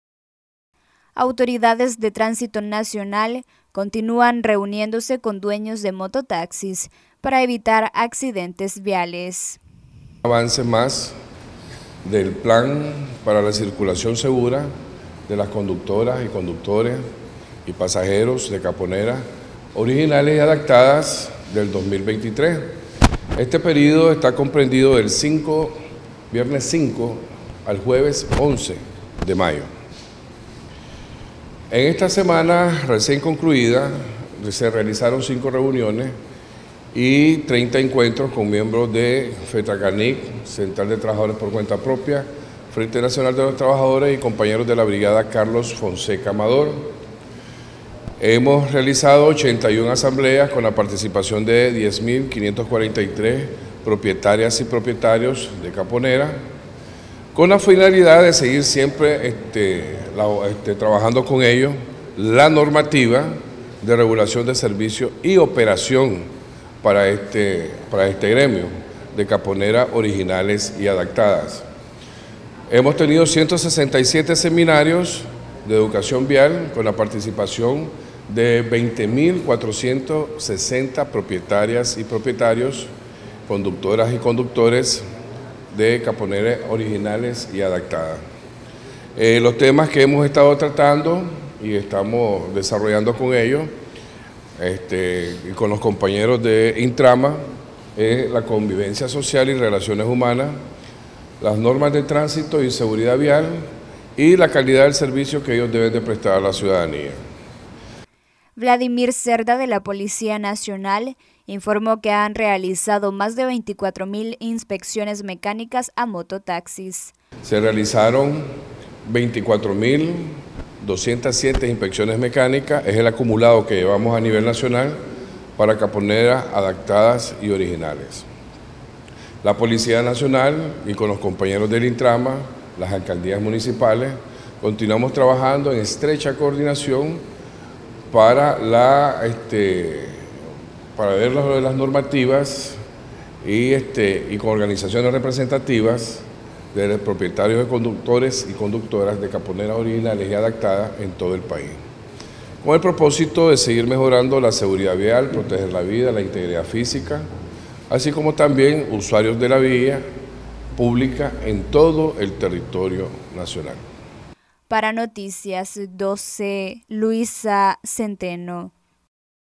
Noticias12Nicaragua Tránsito realiza más de 24 mil inspecciones mecánicas a mototaxis
Tránsito realiza más de 24 mil inspecciones mecánicas a mototaxis